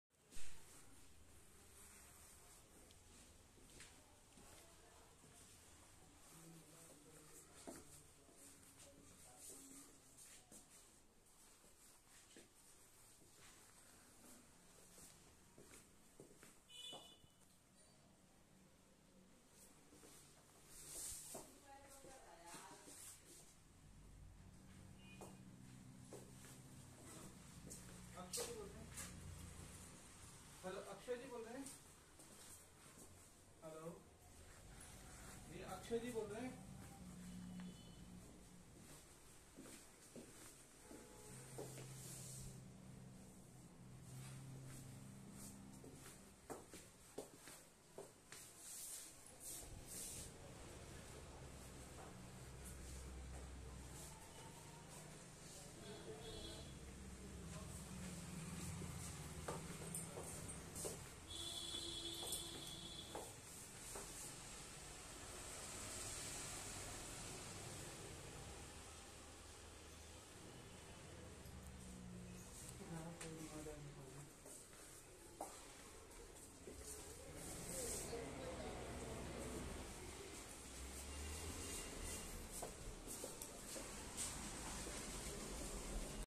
फ्री होल्ड प्रोपर्टी सबसे सस्ते फ्लैट उत्तम नगर में
सबसे सस्ते फ्लैट उत्तम नगर में।
फ्री होल्ड प्रोपर्टी।90% लोन अवेलेबल।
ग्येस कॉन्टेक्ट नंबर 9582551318